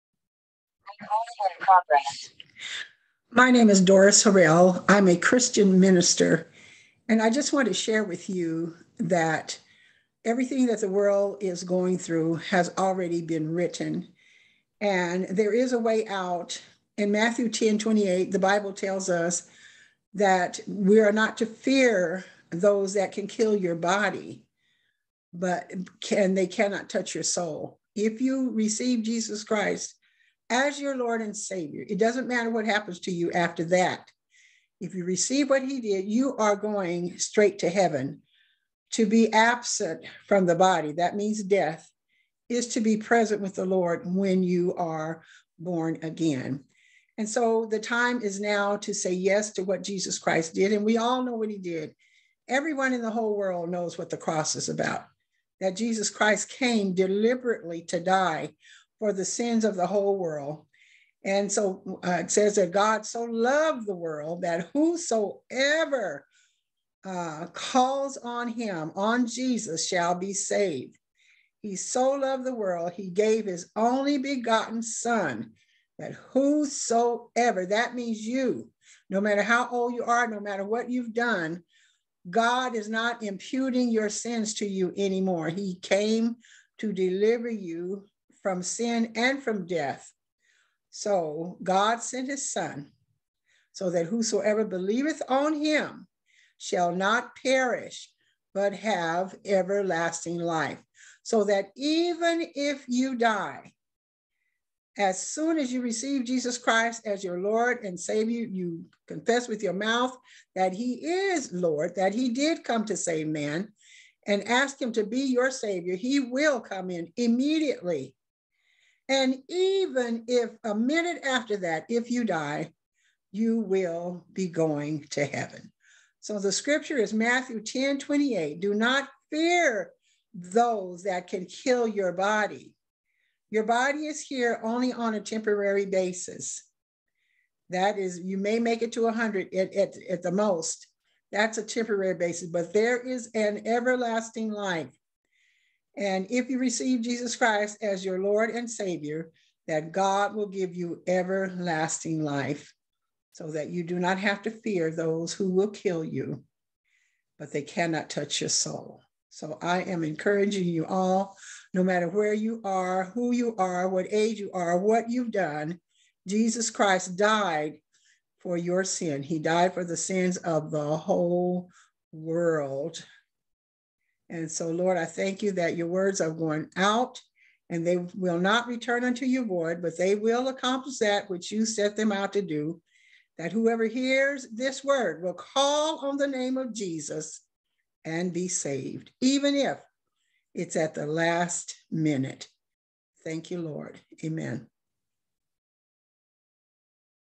AN URGENT END TIME MESSAGE TO ALL WHO ARE IN FEAR WORD-OUT OUTREACH CFTADM
FOR WHOSOEVER SHALL CALL UPON THE NAME OF THE LORD SHALL BE SAVED!    ROMANS 10:13

MATTHEW 10:28
Fear not them which can kill the body. but are not able to kill the soul:

JOHN 3:16
For God so loved the world, that he gave his only begotten Son,
that whosoever believeth in him should not perish,  but shall have everlasting life.

DO IT NOW.  EVEN AT THE LAST MOMENT! CALL ON HIS NAME!
JESUS! JUSUS! JESUS!
HE IS THE SAVIOR OF THE WHOLE WORLD.

DORIS HARRELL
WORD-OUT OUTREACH
CFTADM
Sacramento, California